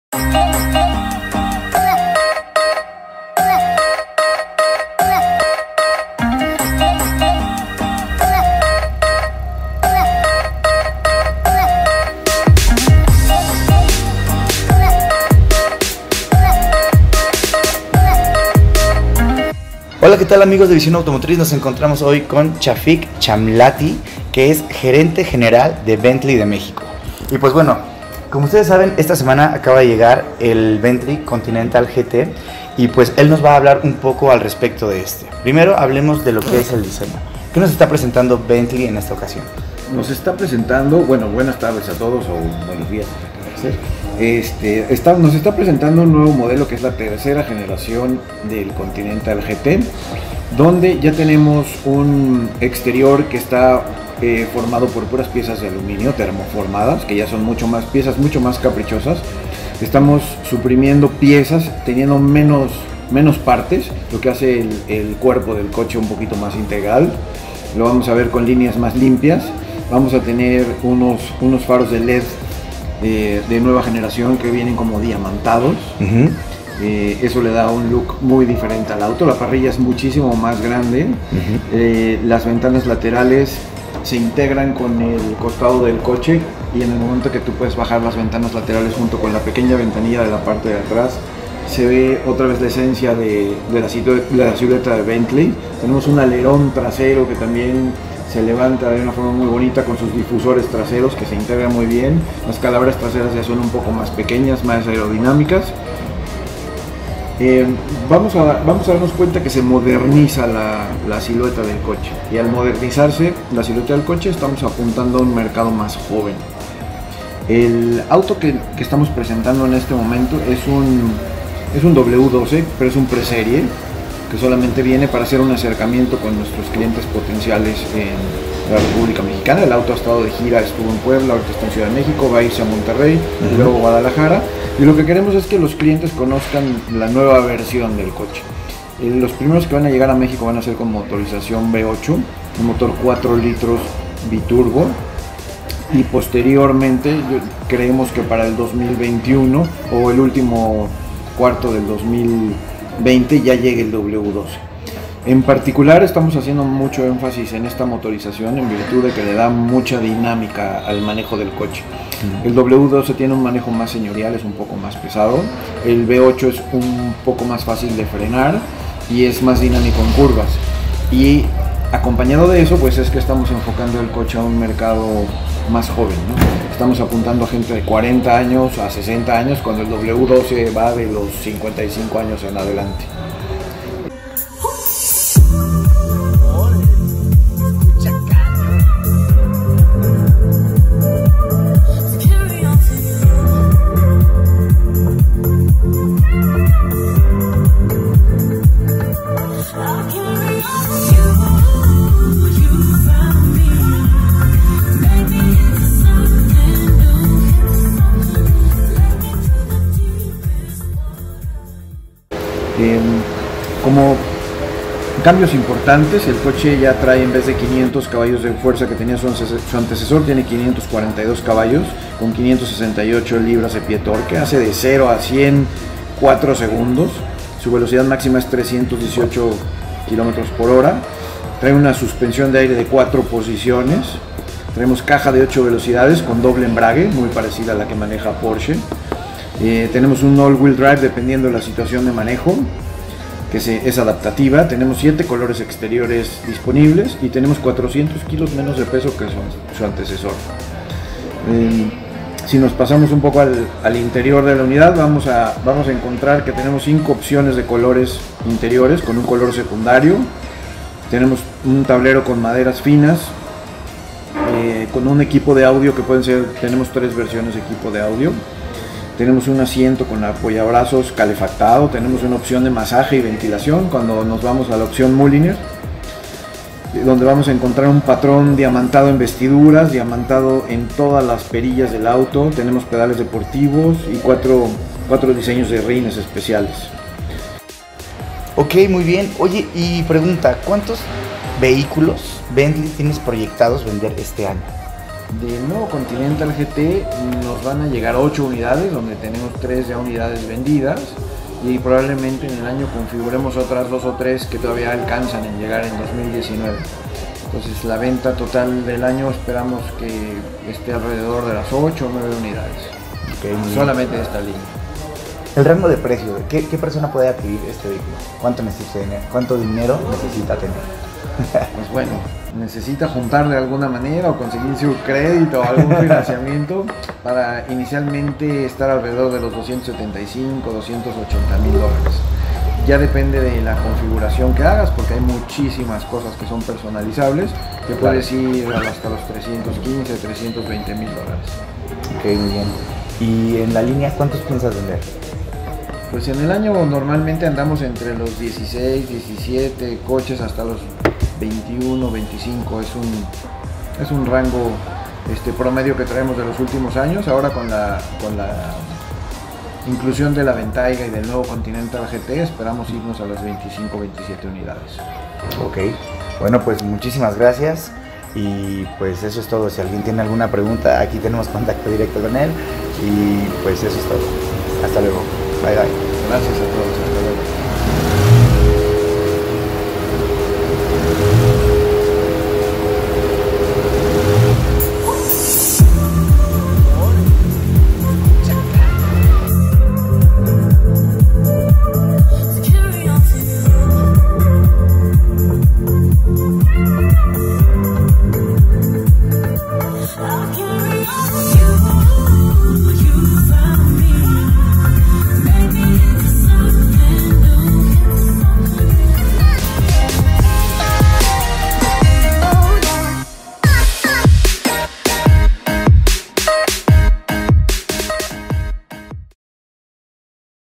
BENTLEY CONTINENTAL GT, ¡ Nunca imaginaras cuánto cuesta y cuántos vehículos venden al año !
El Bentley Continental GT, vehiculo de super lujo, está de gira por Mexico y la cantidad de vehículos que venden al año es sorprendente considerando las marcas más comerciales.

Chafic Chamlati Gerente General de Bentley de México en entrevista nos da todos los detalles del Continental GT y de como va posicionándose la marca en el país (México).